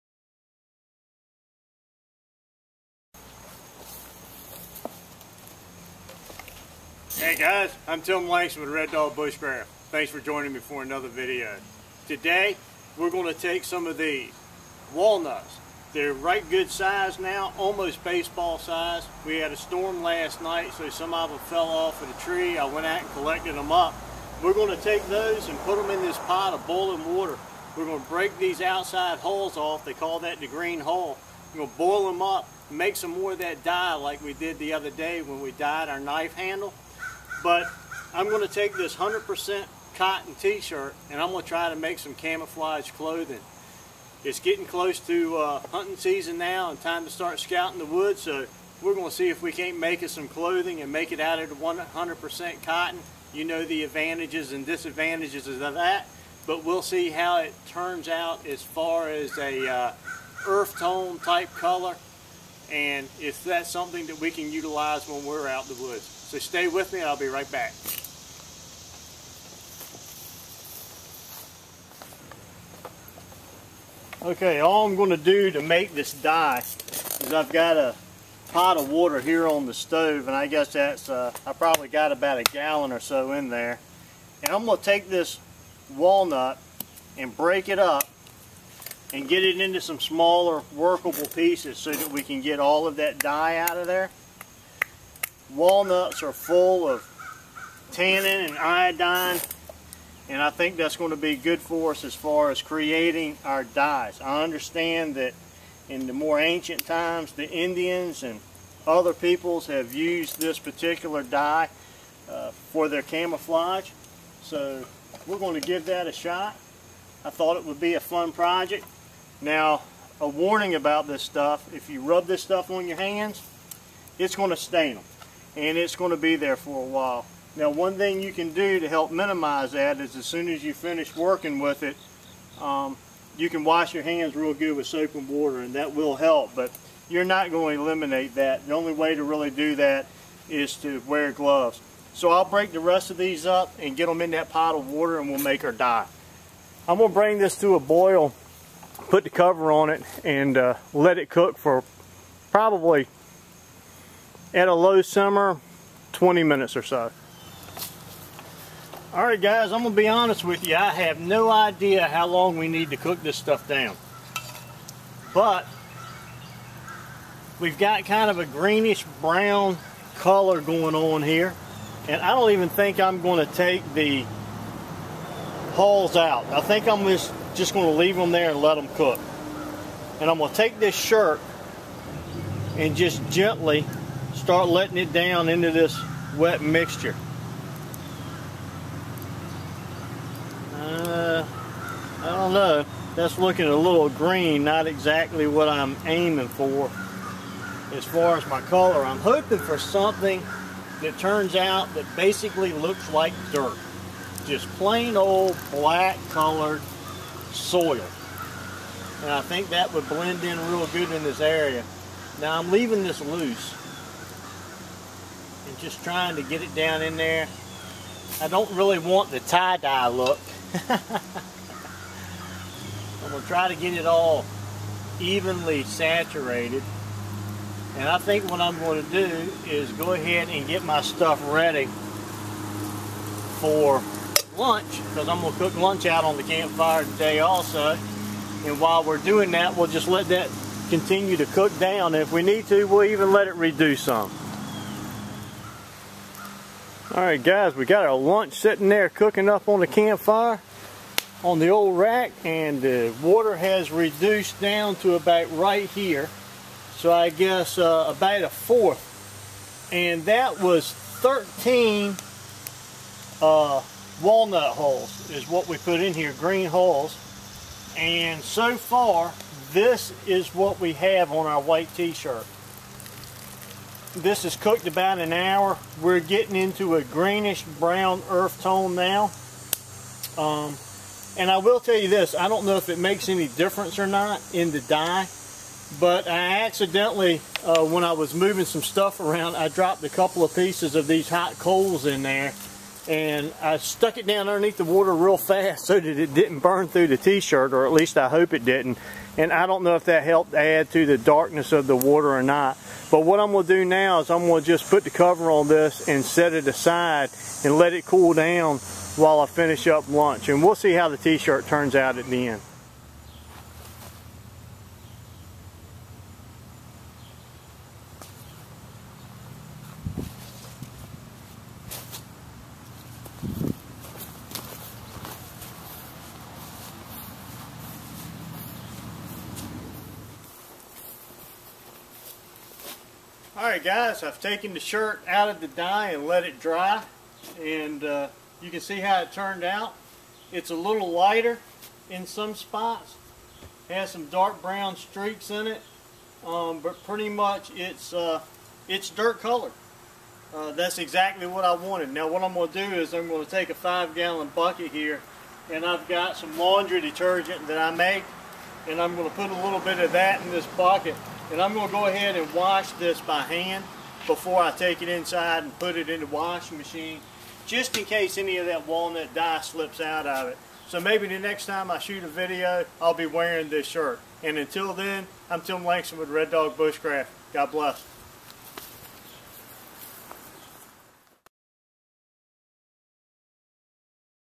Homemade camo shirt with walnut die
I died a shirt dirt color with homemade walnut die.